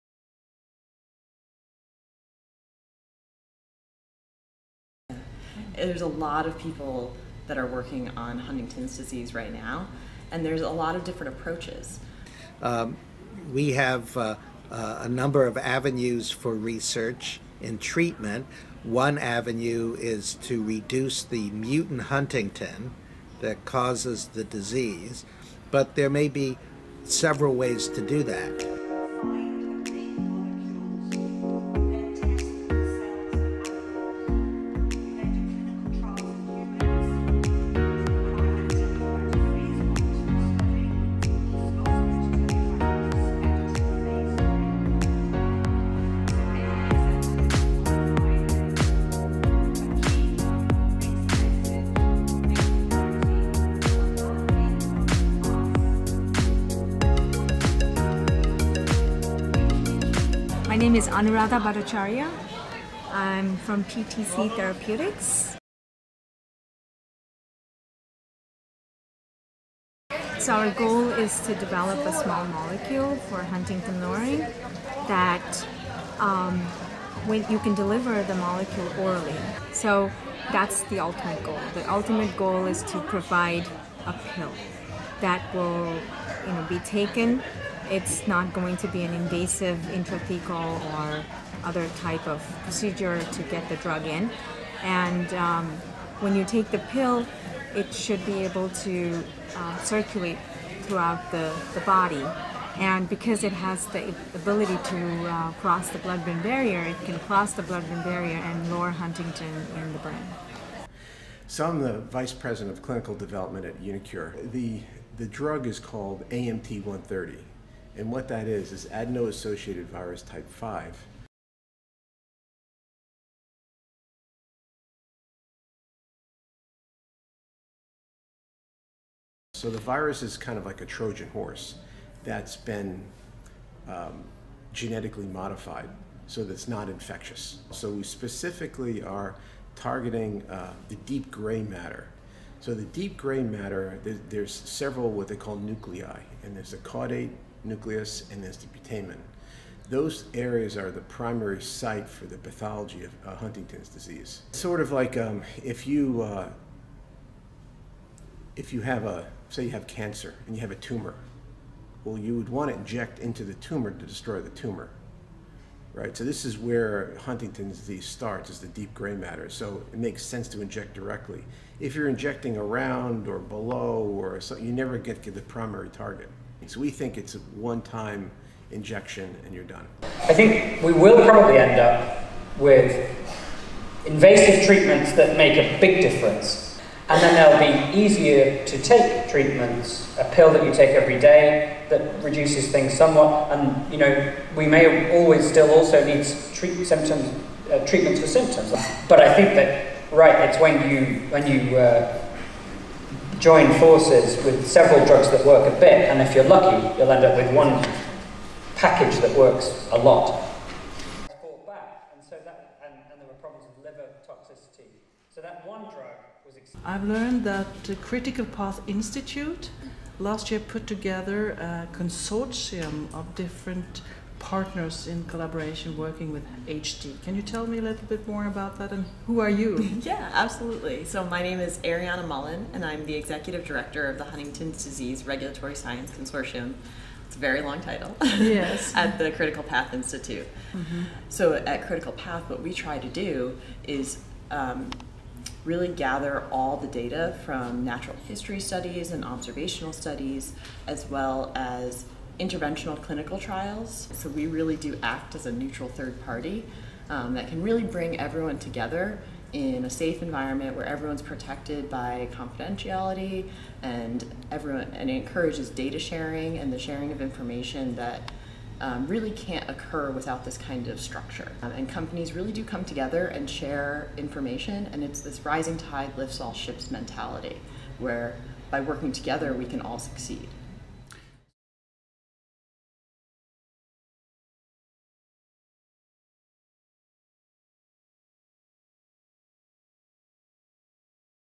Huntington's Disease treatment - Where are we now?
We have talked with different people to get an update on what is currently happening with Huntington’s disease treatment.

At the moment six pharmaceutical companies are working on developing or testing Huntingtin lowering drugs. Two of them are PTC Therapeutics and uniQure. Hear more about their approaches in the video!